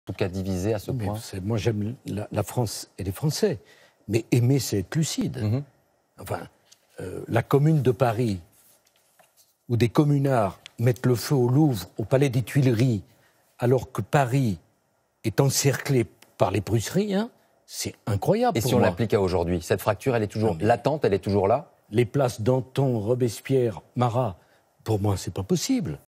Sarkozy et l'Histoire - 30 juin 2019
Un ignorant, et pourtant ancien Président... le siège de Paris par les prussiens se termine le 26 janvier 1871, la Commune démarre le 18 mars... le Louvre est incendié le 24 mai, pendant la Semaine Sanglante, alors que Bismarck a libéré 60.000 soldats français prisonniers pour rejoindre les versaillais de Thiers qui ne disposait que de 12.000 hommes... et si pour cet ignare "Danton, Robespierre, Marat, c'est impossible", alors pour lui c'est la République qui est impossible... Dont acte !